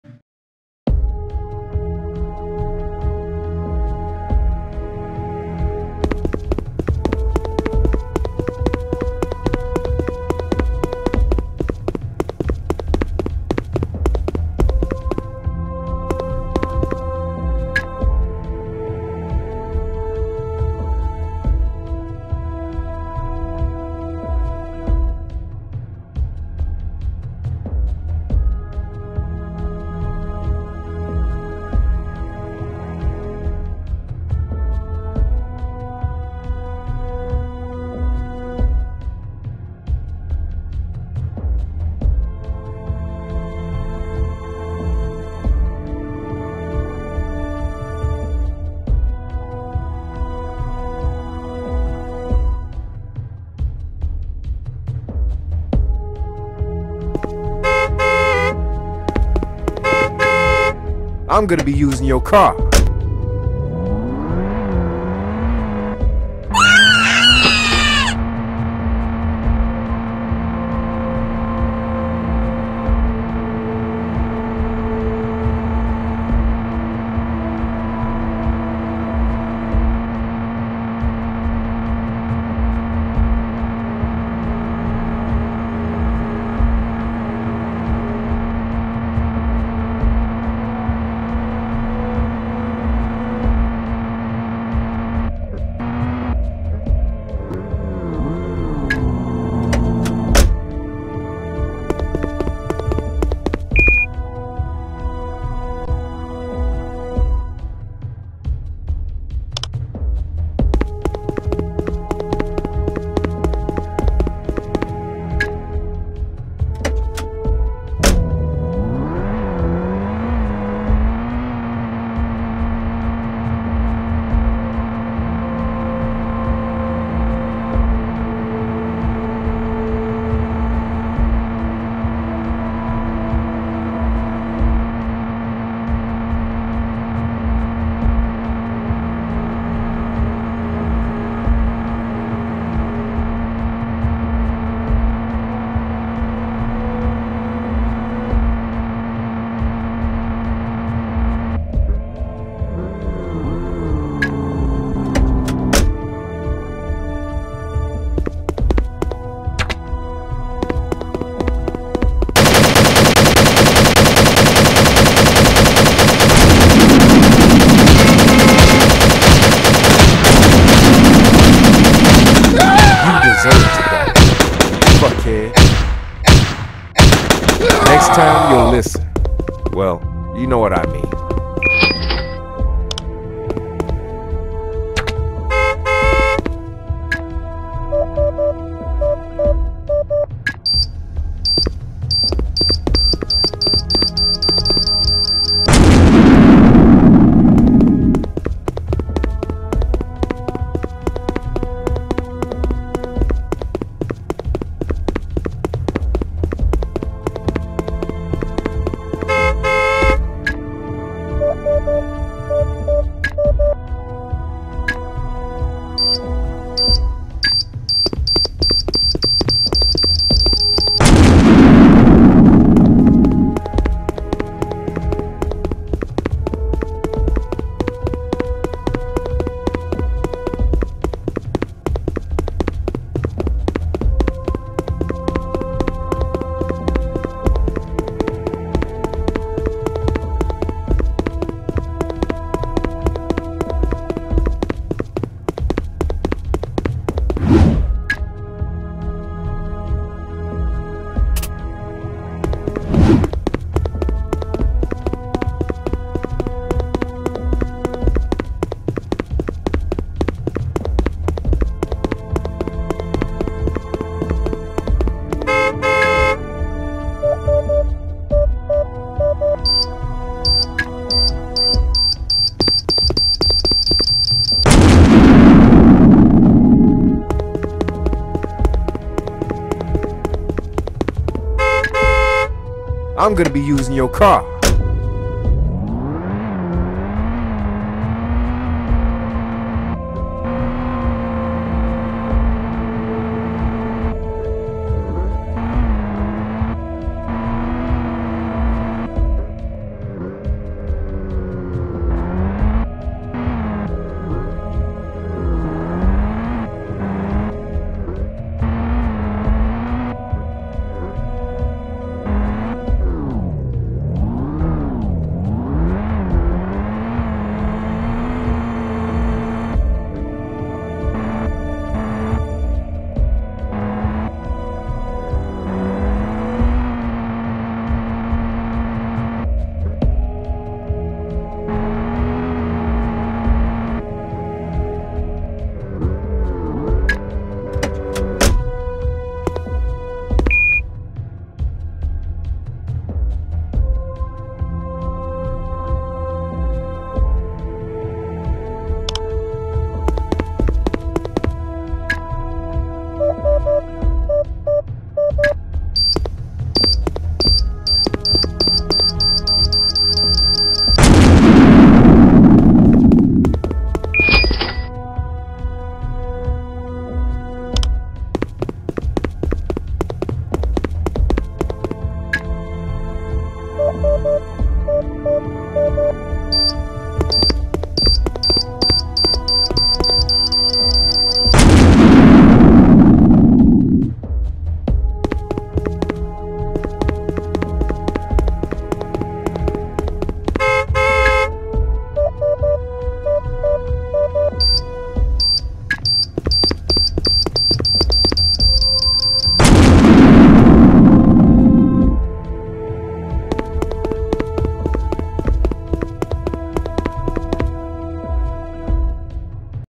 Miami 3D City | Destroying Expensive Cars
Blasting Expensive cars in Miami with time bomb